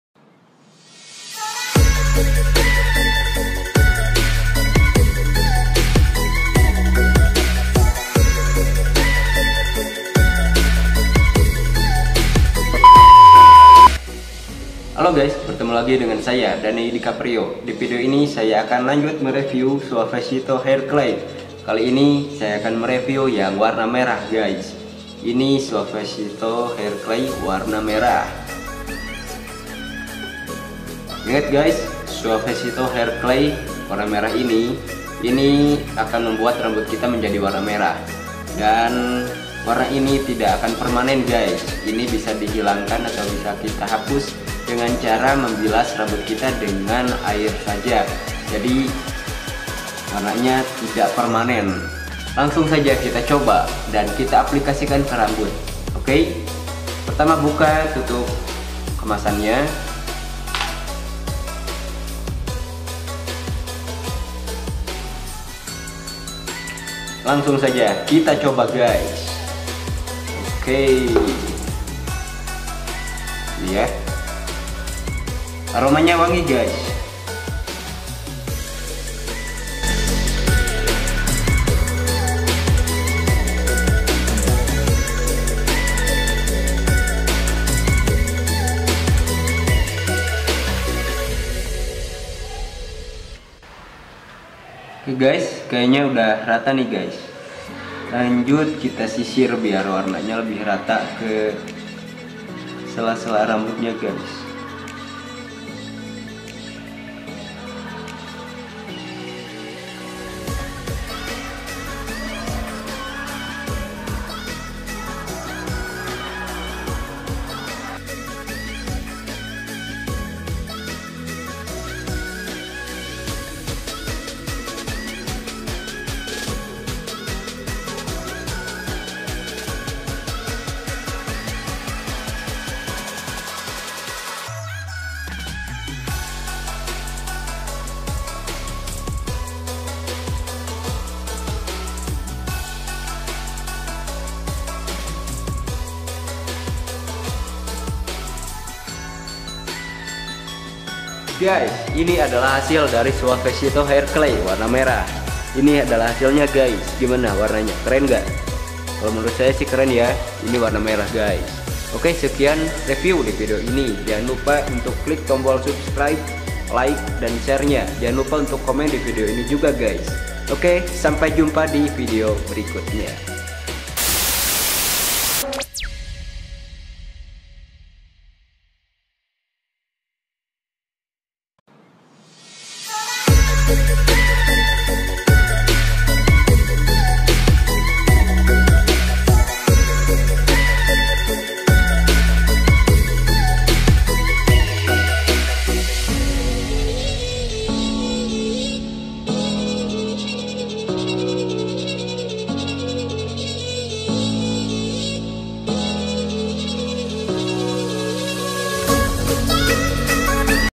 POMADE WARNA MERAH, SUAVECITO RED HAIR CLAY, KEREN BROOO
Divideo ini saya mereview Suavecito Red Hairclay,
bagi kalian yang suka gunta ganti gaya rambut dan warna rambut, kalian harus coba ini, karena suavecito adalah hairclay yang bisa merubah gaya dan warna rambut kalian secara cepat dan aman, dan ini tidak permanent, hanya dengan membilasnya dengan air, warna pada rambut akan mudah dibersihkan dan dihilangkan.

lebih jelasnya lagi tonton terus video ini sampai habis ya guys..